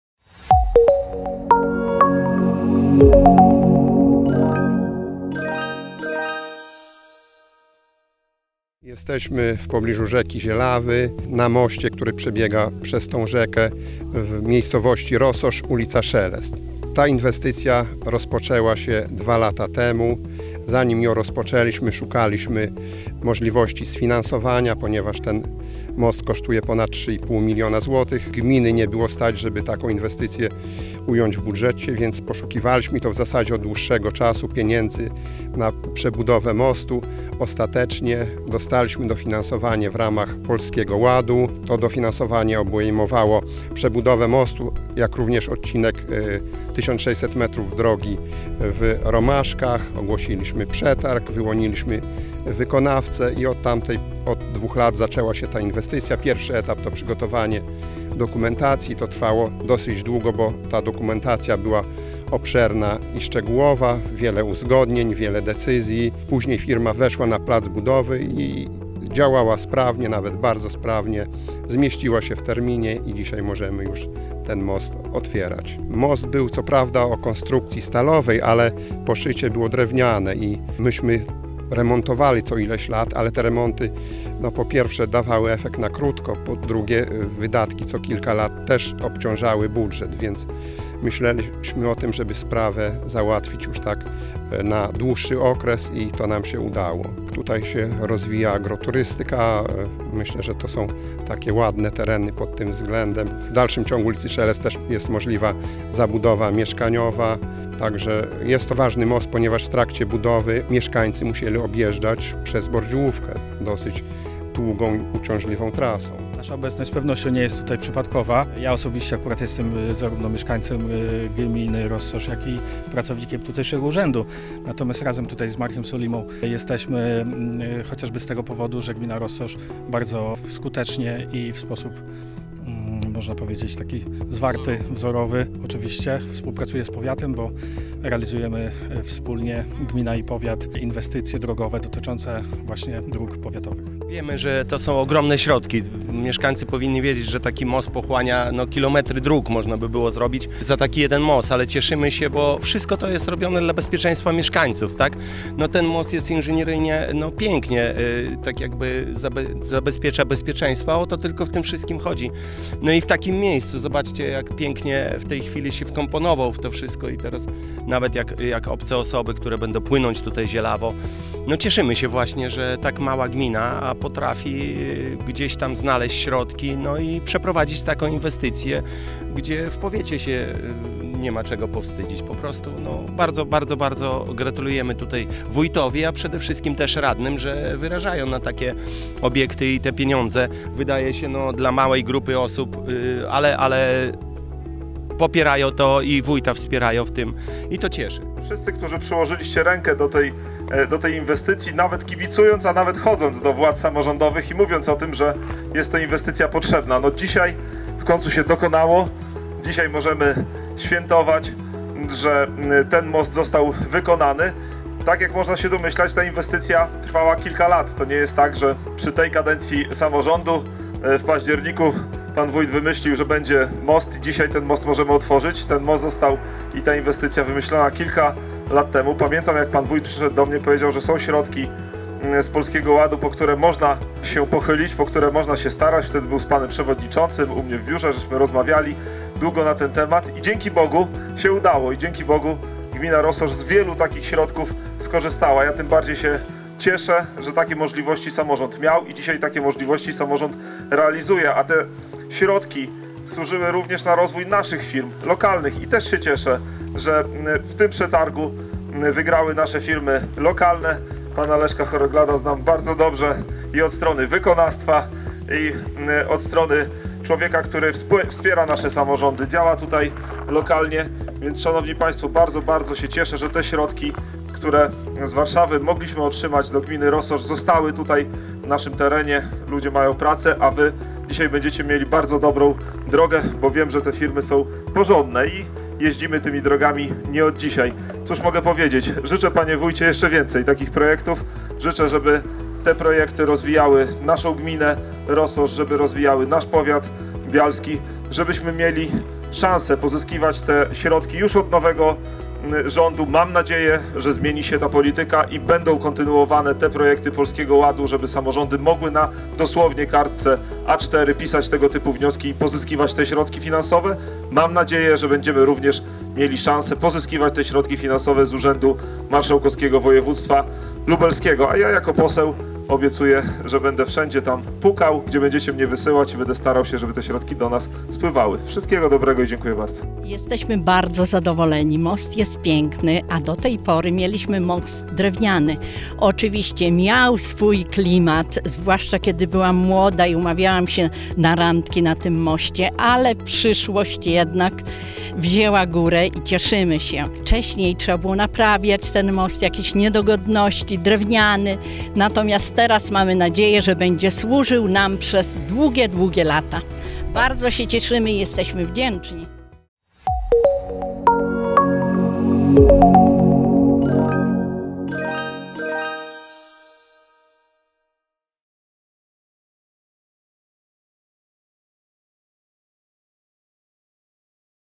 Rossosz z nowym mostem. Zbudowany został w ulicy Szelest
Zadanie prowadzono na 22 metrach ulicy Szelest. To wschodnia część Rossosza. Sam most połączył dwa brzegi Zielawy.